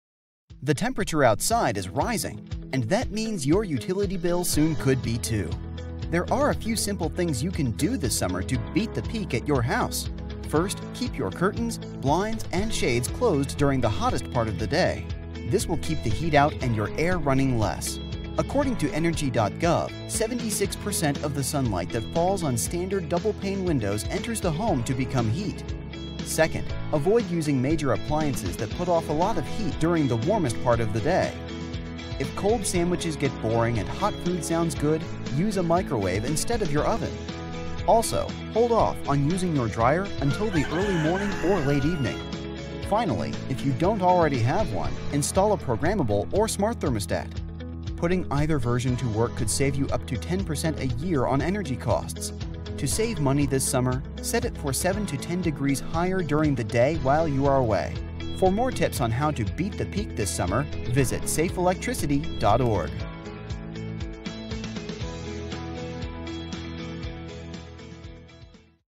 Beat the Peak with the Easy Button
According to the Department of Energy, a typical home uses a whopping 48 percent of energy expenditures just on the heating, ventilation and air conditioning system (HVAC) system.  Although a majority of that 48 percent is spent on heating expenses, Americans still spend $29 billion every year to power their air conditioners.